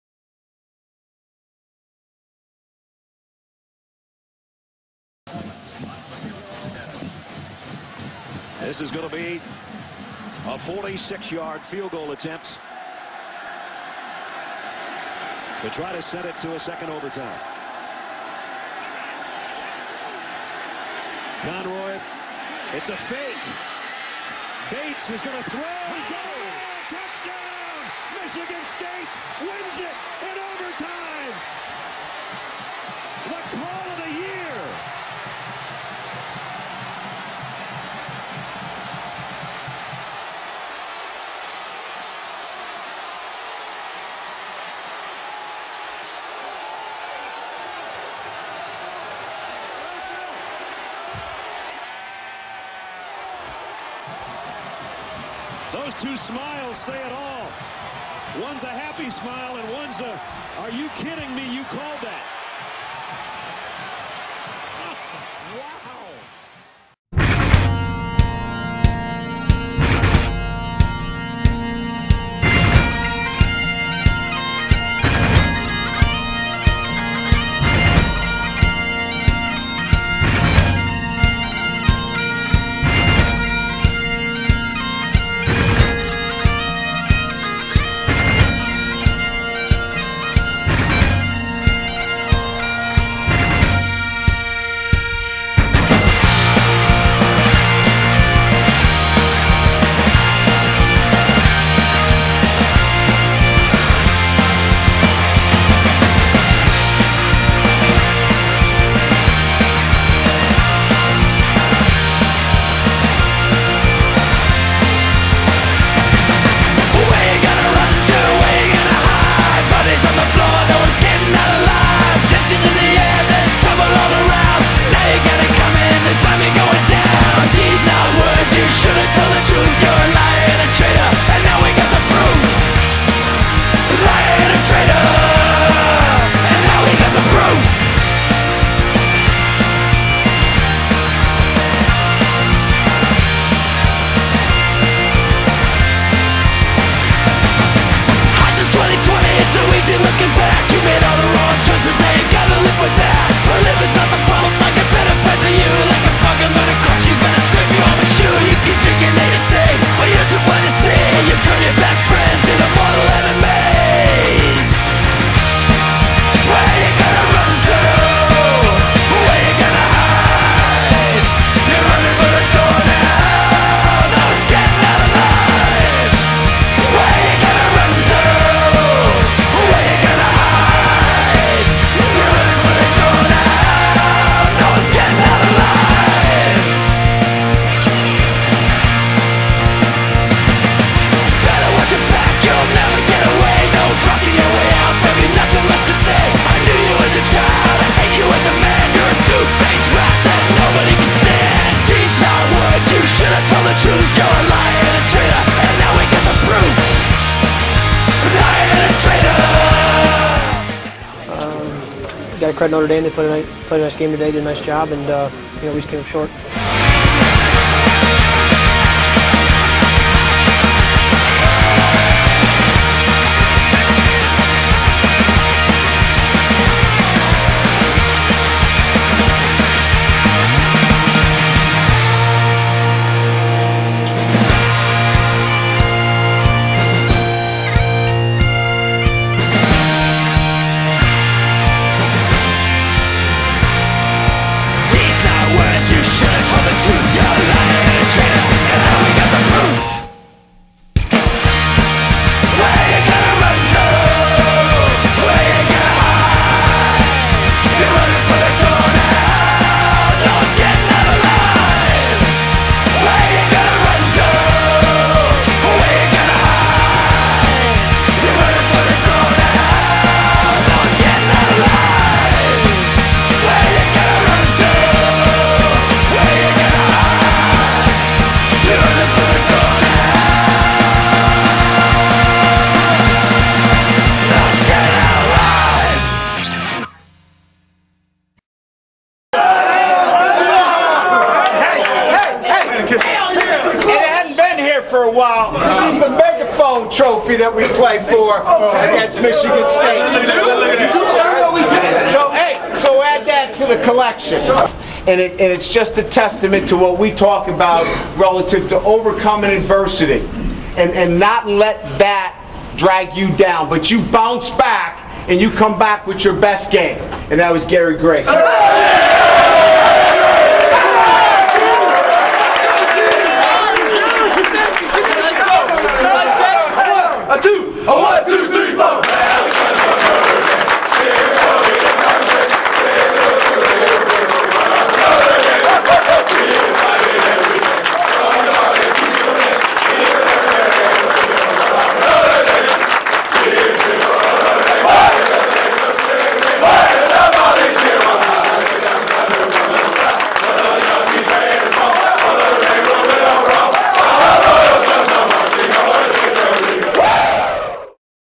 Michigan State vs. Notre Dame 2011: ND 31 - MSU 13
Notre Dame Upends the Spartans for a resounding first Victory in 2011. GO IRISH!!